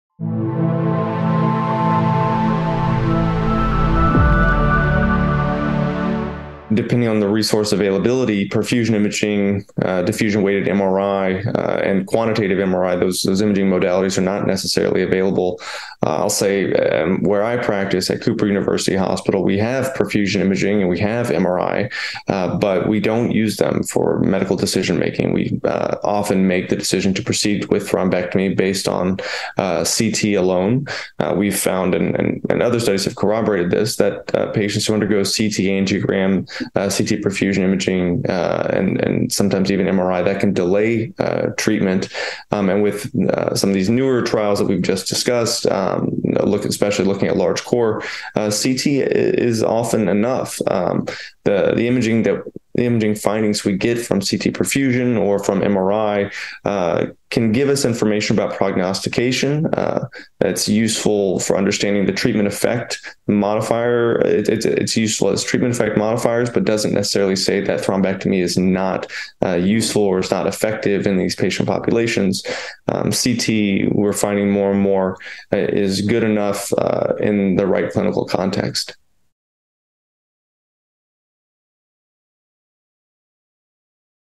Using advanced imaging in daily stroke practice
James E. Siegler, MD, Cooper Neurological Institute, Cooper University Hospital, Camden, NJ, shares his thoughts on the value of advanced imaging for patient selection in acute ischemic stroke management. Imaging protocols vary across centers, according to resource availability, with CT perfusion and diffusion weight MRI often being unavailable. Dr Siegler notes that while advanced imaging can provide useful prognostic information on potential treatment effect modifiers, its use is not typically necessary for medical decision making. This interview took place during the International Stroke Conference (ISC) 2023 in Dallas, TX.